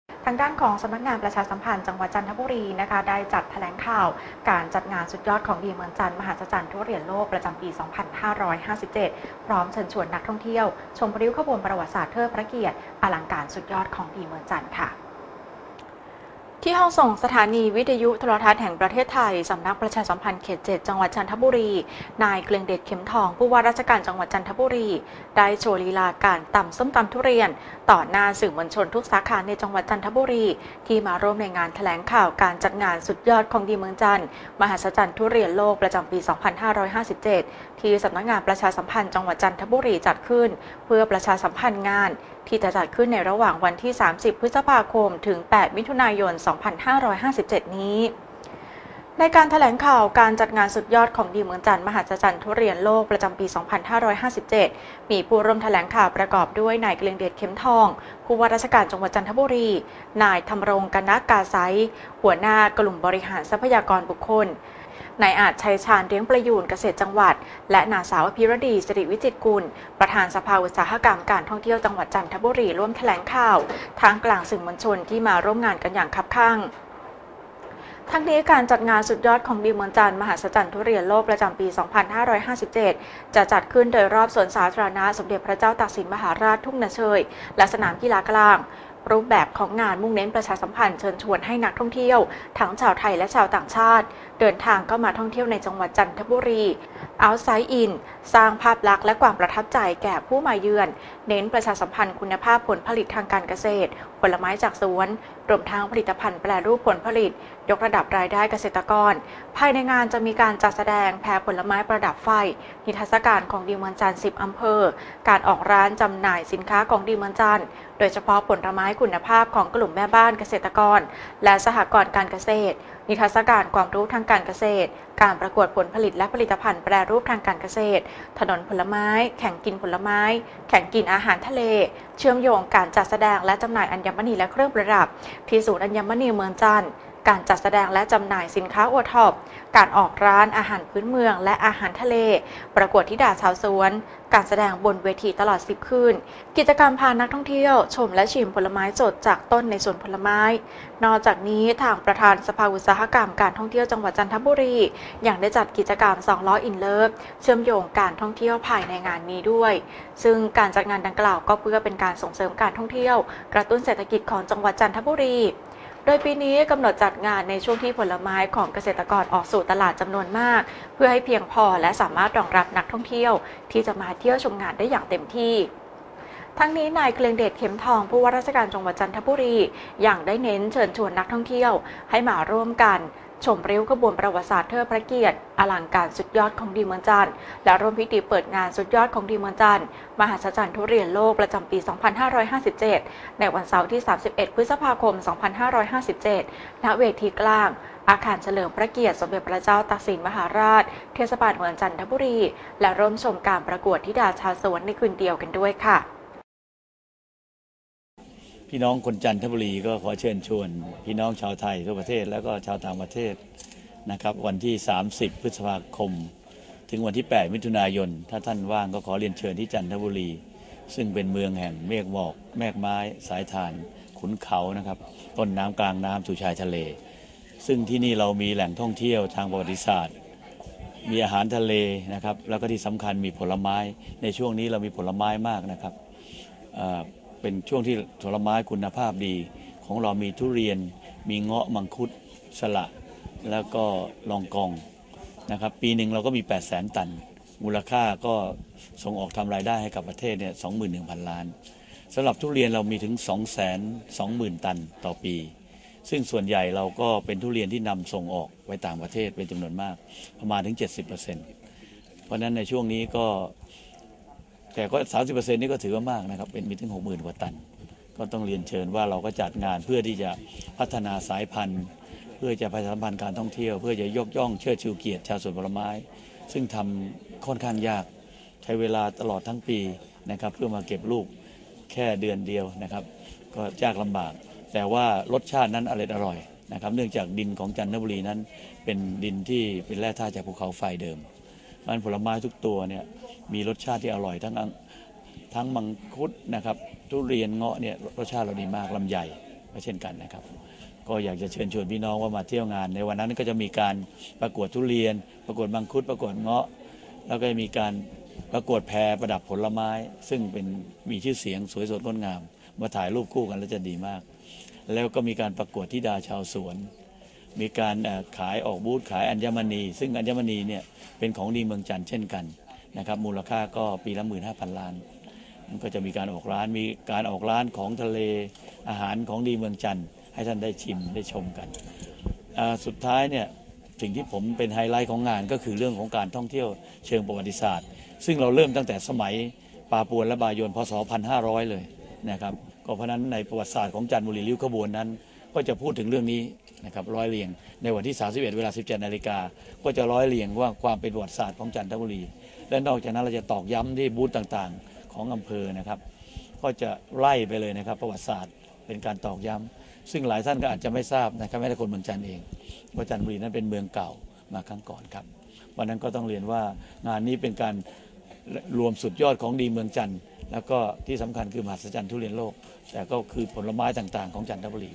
21-5-57 แถลงข่าว งานสุดยอดของดีเมืองจันท์ มหัศจรรย์ทุเรียนโลก
สำนักงานประชาสัมพันธ์จังหวัดจันทบุรี จัดแถลงข่าว การจัดงานสุดยอดของดีเมืองจันท์ มหัศจรรย์ทุเรียนโลก ประจำปี 2557 พร้อมเชิญชวนนักท่องเที่ยวชมริ้วขบวนประวัติศาสตร์เทิดพระเกียรติฯ อลังการสุดยอดของดีเมืองจันท์